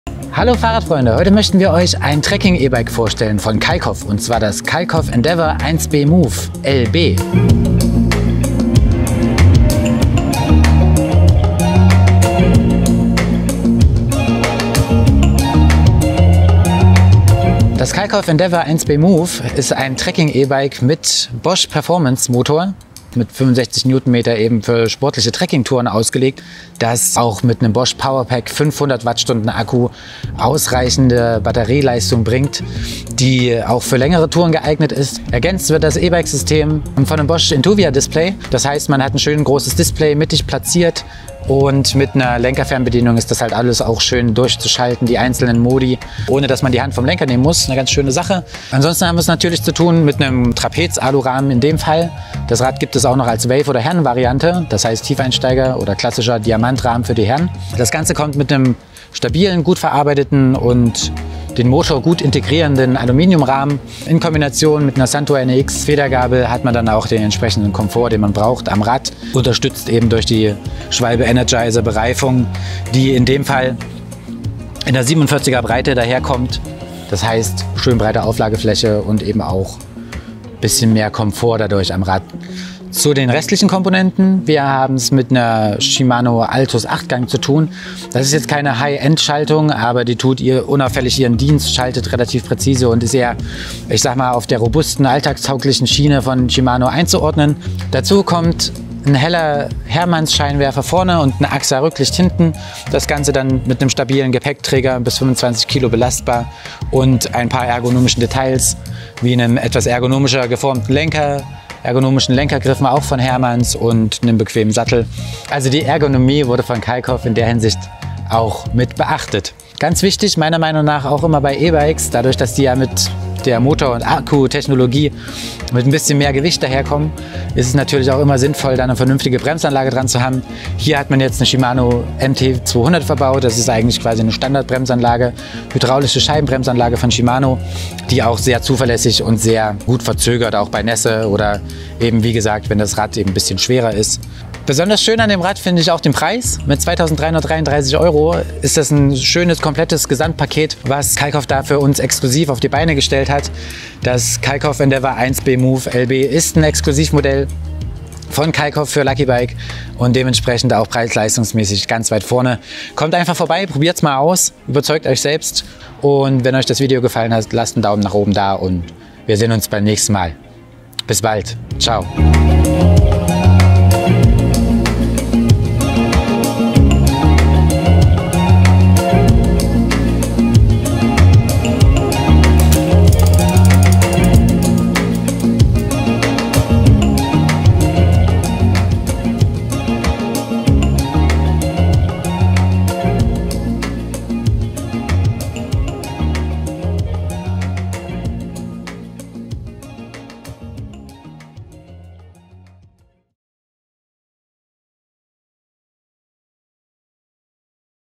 Produktvorstellung Kalkhoff Endeavour 1.B Move LB 2020 | Exklusiv bei Lucky Bike!
Das exklusive Kalkhoff Endeavour 1.B Move LB Trekkingpedelec wartet mit Eleganz im Rahmendesign und kraftvollen Bosch E-Antriebskomponenten zu einem exzellenten Preis-/Leistungsverhältnis auf.
Ideal für lange Touren und den täglichen Pendlereinsatz, zeigt sich das elektrische Antriebssystem von Bosch. Der 65 Nm leistende Performance Line Motor lässt Sie auch stärkstes Gefälle mühelos erklimmen. Dazu präsentiert Ihnen der große Intuvia Bordcomputer übersichtlich alle fahrtrelevanten Daten zentral am Lenker. Damit Sie Ihre Hände zur Bedienung am Lenker lassen können, ist das Intuvia Display zusätzlich mit einer Fernsteuerung ausgestattet. Komplettiert wird das moderne Bosch Paket von einem 500 Wh leistenden PowerPack Akku, der sich dank der Montage am Unterrohr angenehm einfach entnehmen lässt. Die Kombination aus Performance Line Mittelmotor und 500 Wh Akkuleistung lässt Sie hohe Reichweiten erzielen und bietet sich ideal für ausgedehnte Trekkingtouren und den täglichen Pendlereinsatz an.